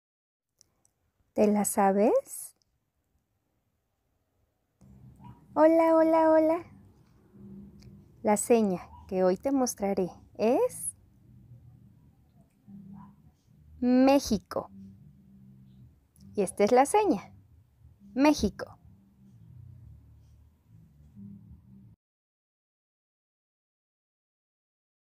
¿Te la sabes? "México" (LSM)
Sigamos aprendiendo en nuestra sección: ¿Te la sabes?

La seña que hoy mostraremos es: "México"